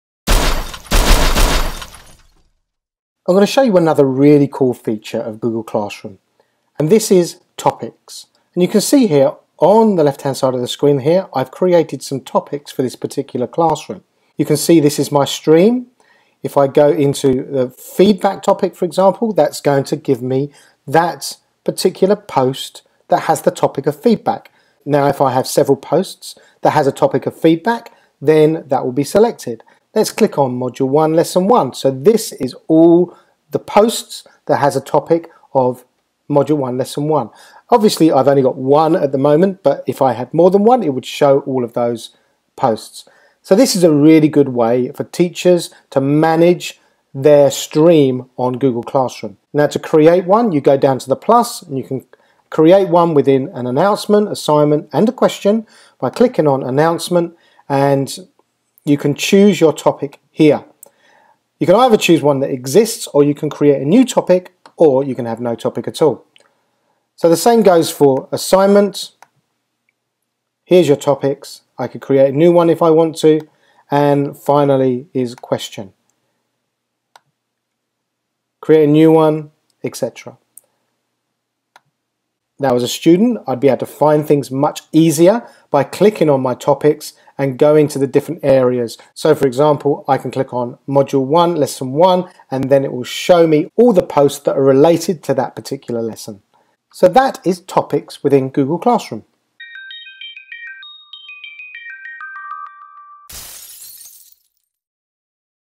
The Coolest Features of Google Classroom: Organise your Stream by creating and using topics - Topics
One of the big problems with Google Classroom has been the lack of being able to find different posts on your Stream. Now Google Classroom offers a feature called Topics that allows teachers to tag a poster with a topic so that students and teachers can find posts more easily. Topics groups posts together, which makes the job of finding things in the stream so much easier.

Other Great Videos from AtTechEdu